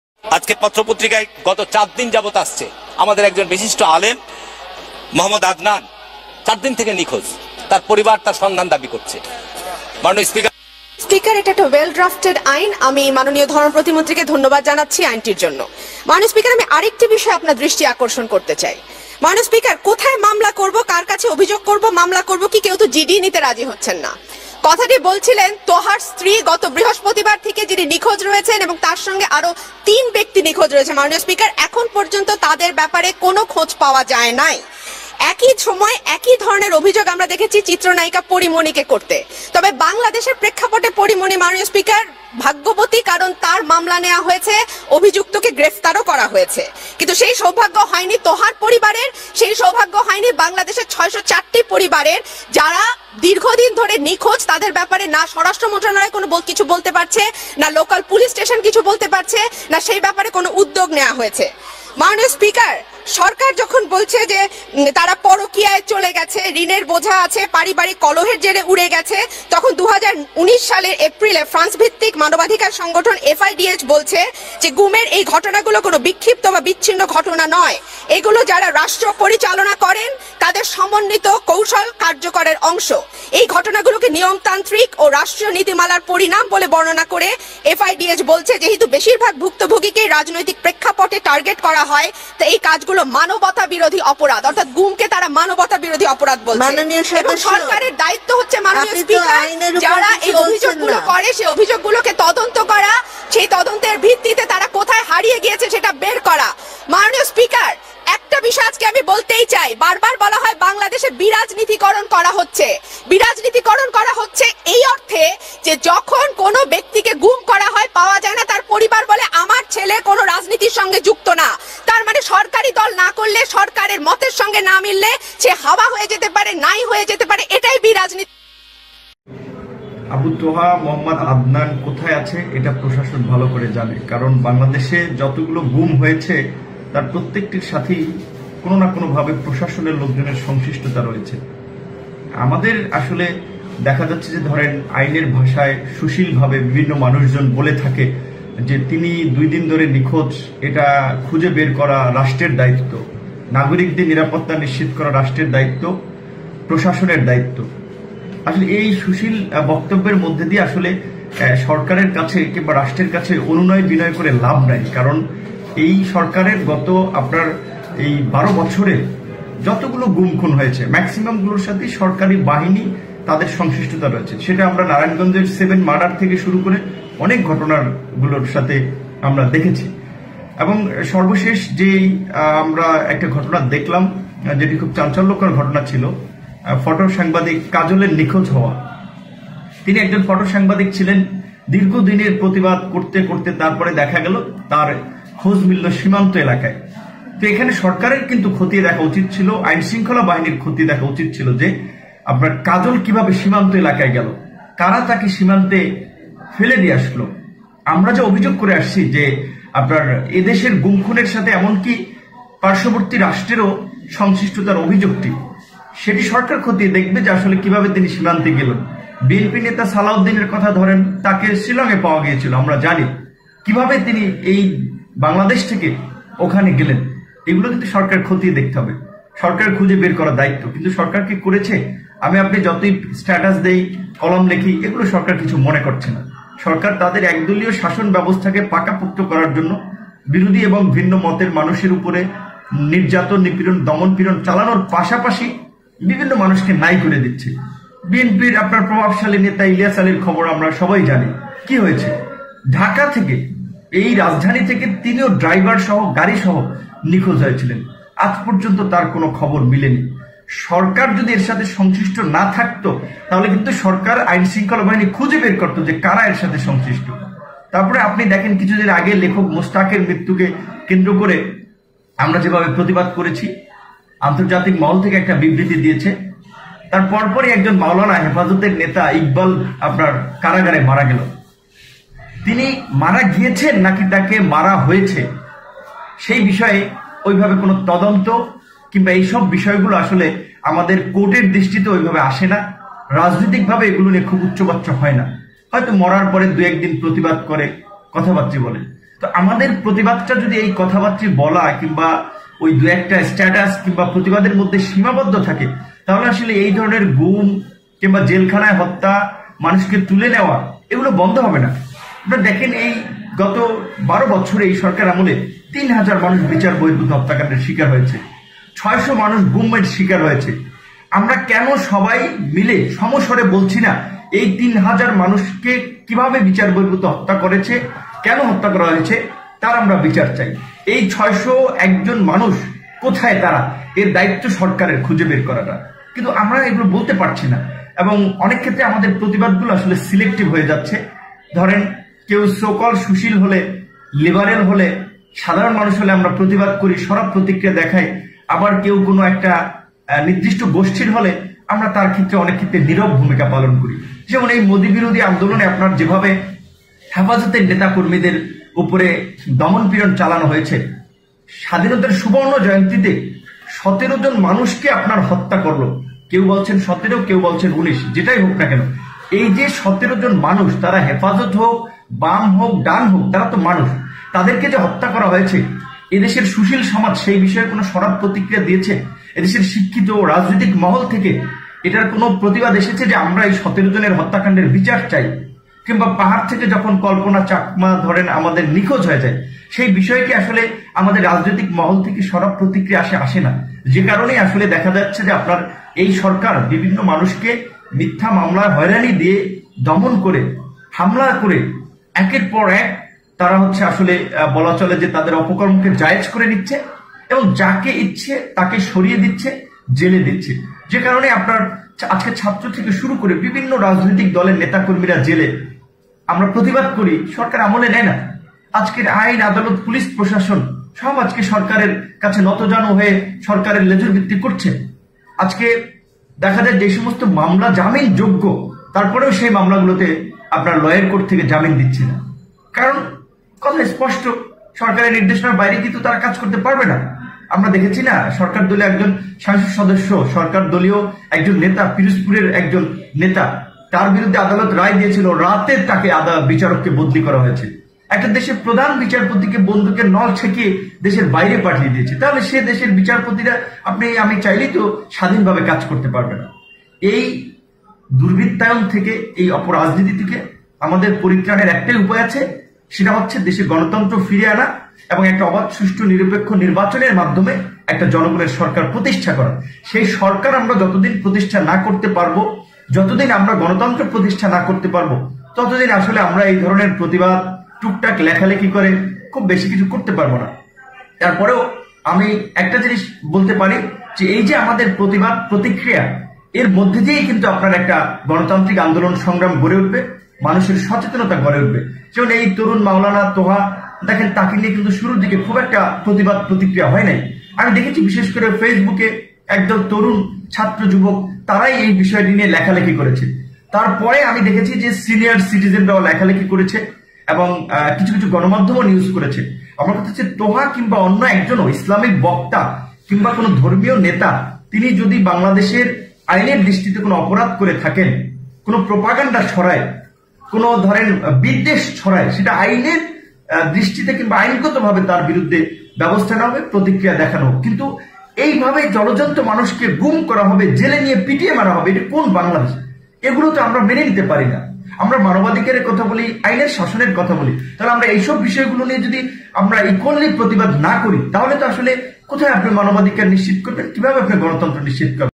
আবু ত্বহাকে নিয়ে সংসদে বিএনপির অগ্নি ঝরা বক্তব্য .abu taha adnan.vp nur.wide islamic tv
আবু ত্বহাকে নিয়ে সংসদে বিএনপির অগ্নি ঝরা বক্তব্য শুরুসংসদে বিএনপিদের জলাময়ি বক্তব্য abu taha adnan